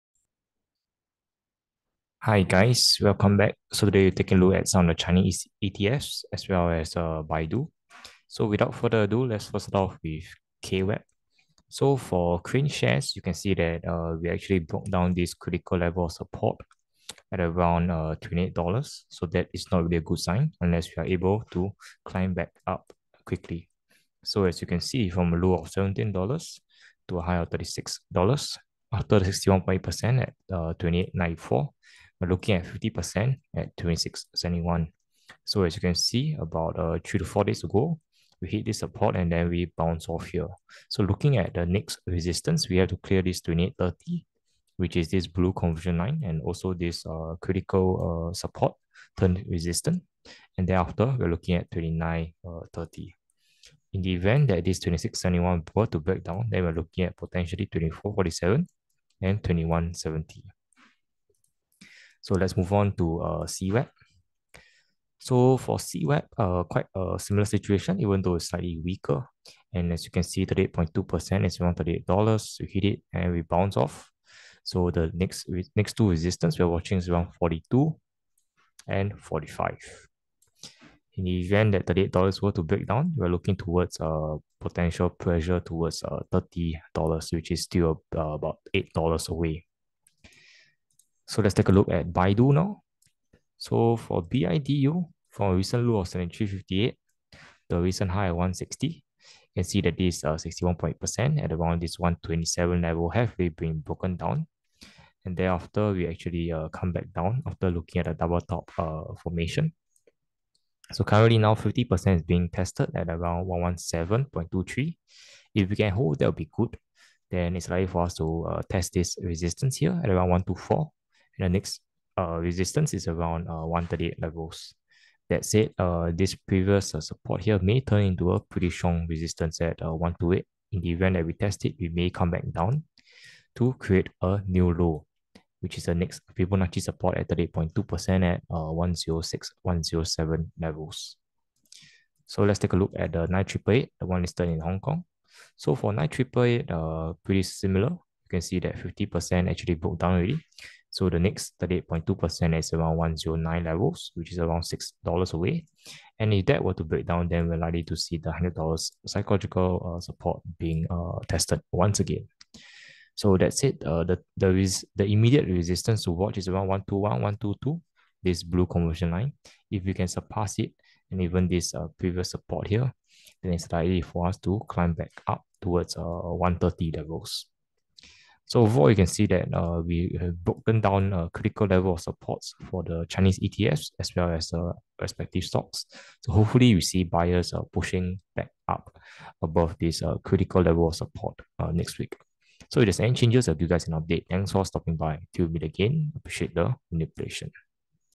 KraneShares Trust ETF KWEB CWEB Baidu 9888 百度 BIDU HongKong 30Apr23│Technical Analysis
Feel free to drop any ticker codes, will analyse them in the next video!